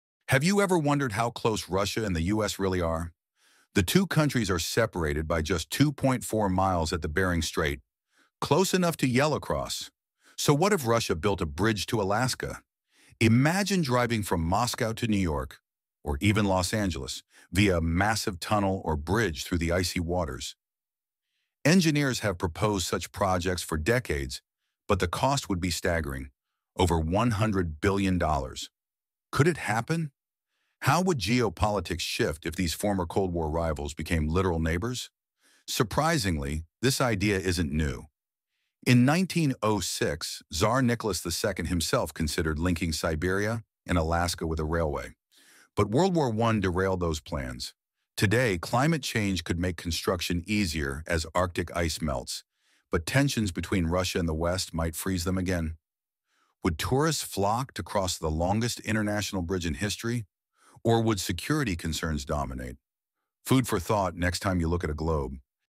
What If Russia Built a Bridge to Alaska? The Bering Strait Bridge Project Explained 🌎
Could a bridge really connect Russia and Alaska? It may sound impossible, but the two countries are only 2.4 miles apart at the Bering Strait!

The Bering Strait Bridge has been proposed multiple times, but would it actually work?

In this video, we explore:

✅ The history of Russia's Alaska bridge proposals 🌍
✅ The cost & engineering challenges of building the Bering Strait crossing 🚧
✅ The geopolitical consequences-Would it change Russia-US relations? 🏛️
✅ The climate change impact and future of Arctic infrastructure ❄️
✅ Could people walk from Russia to Alaska over a frozen Bering Strait? 🤯

Would a Russia-Alaska bridge be a global game changer or an impossible dream? Let's find out!